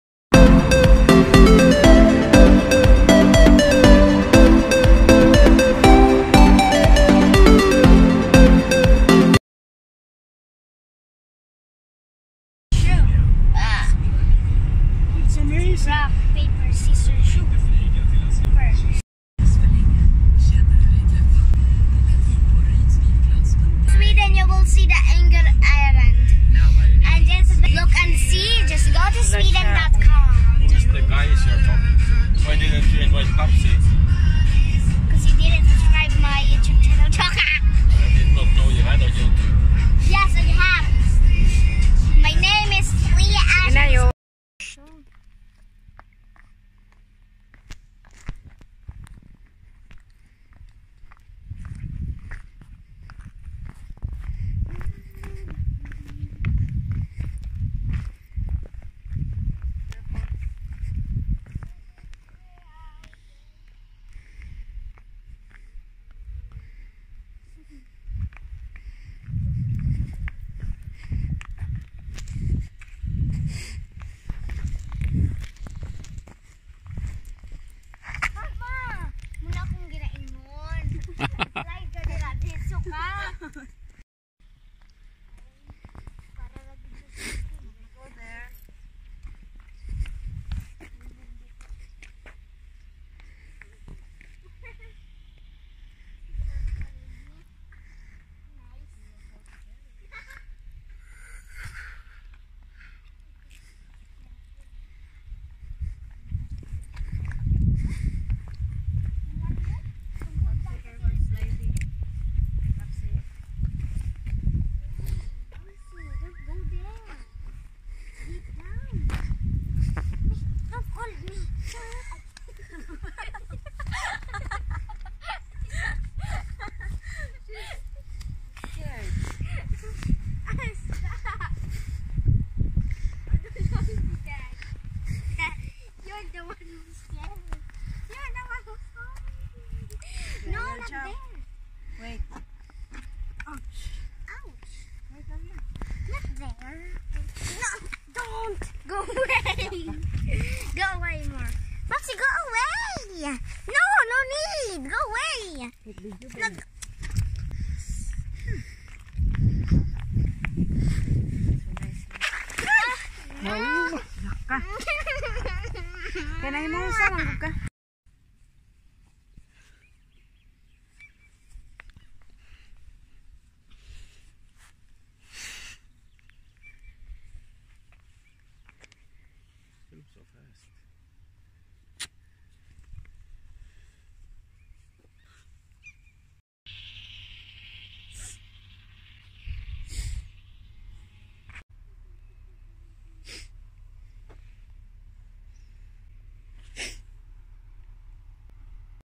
Stockholm adventure : Our first day out Exploring Sweden summer season 2019
some compilations of our vacation